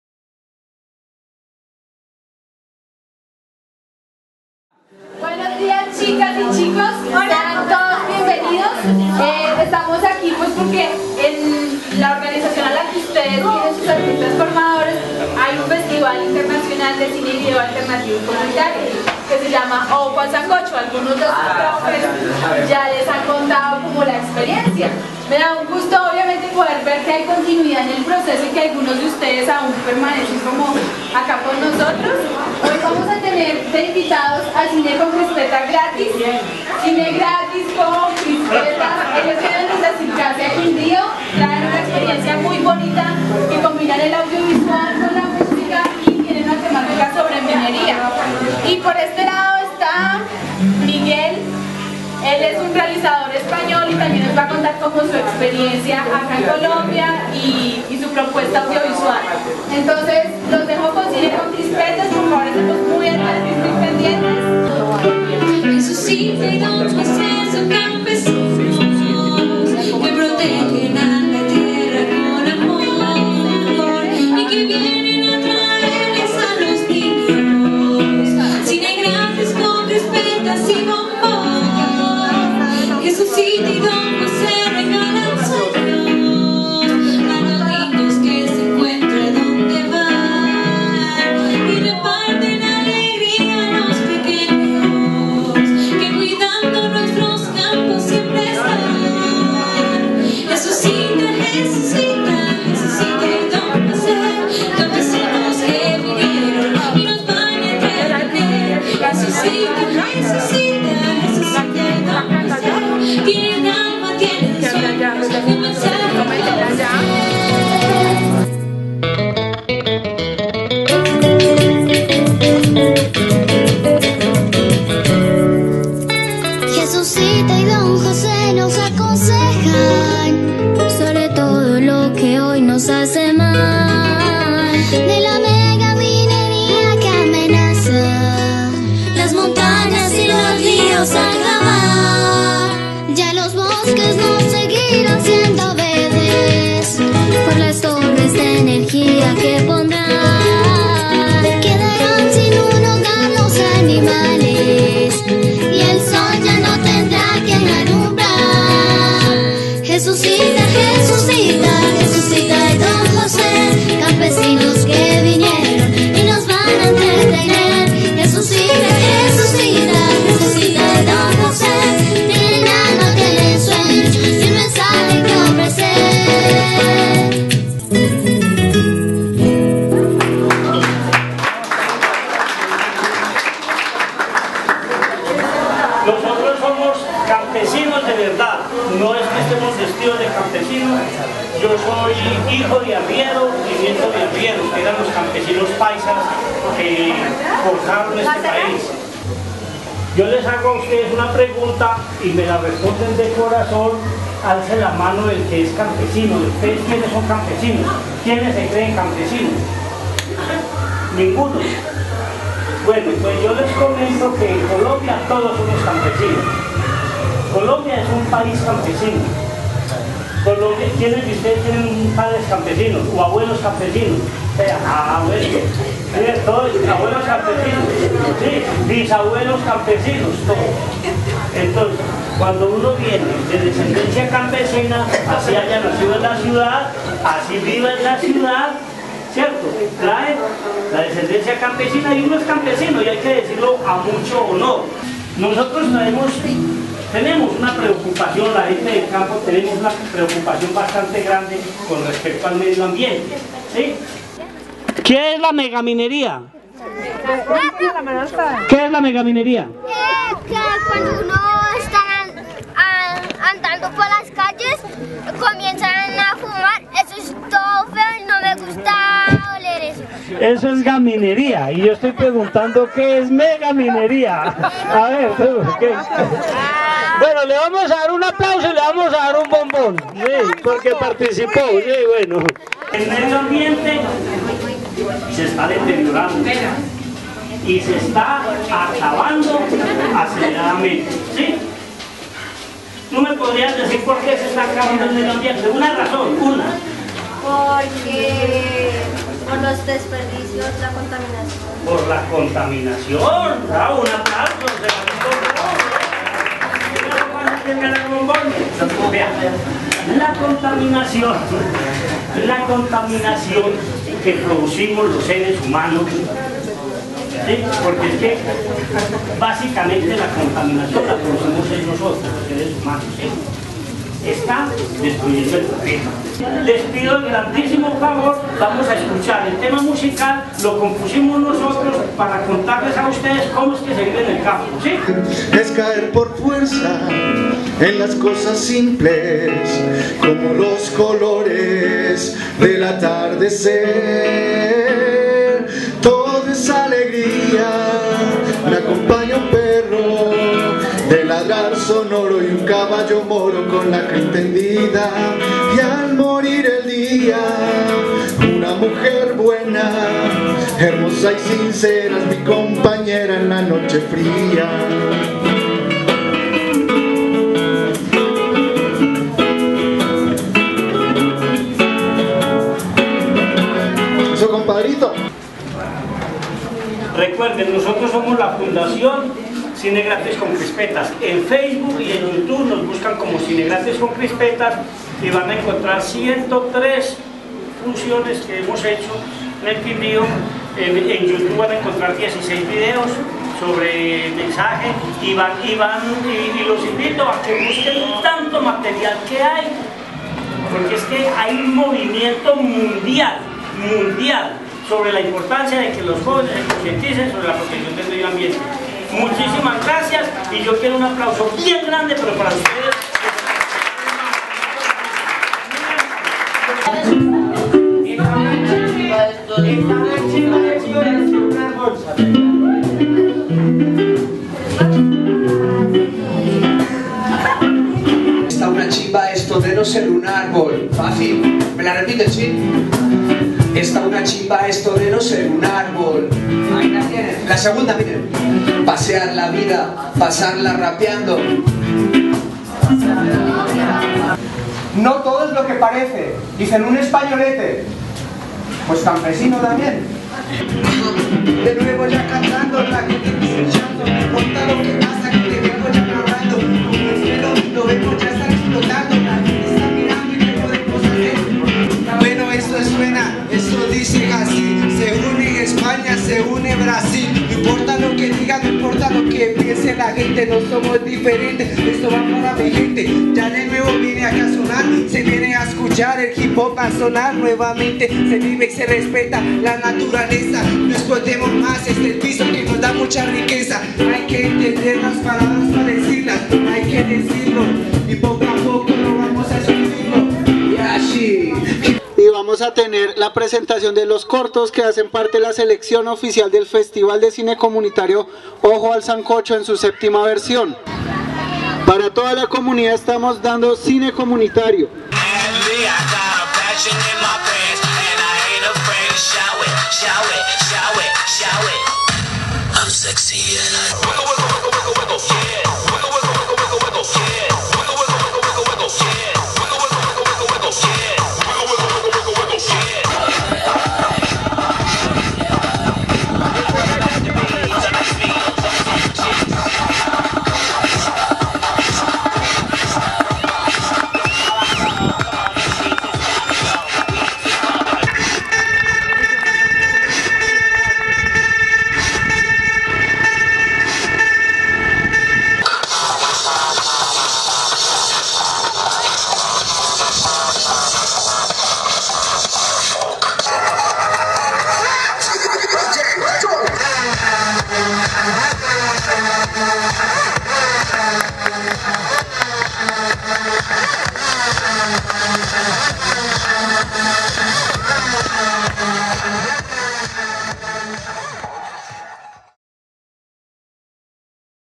Cine Gratis Con Crispeta en el 7º Festival de cine Comunitario Ojo al Sancocho
Con Cine Gratis Con Crispeta
Avanza la Programación Preliminar
del 7º Festival  Internacional
de Cine, video Comunitario
y Alternativo
Ojo al Sancocho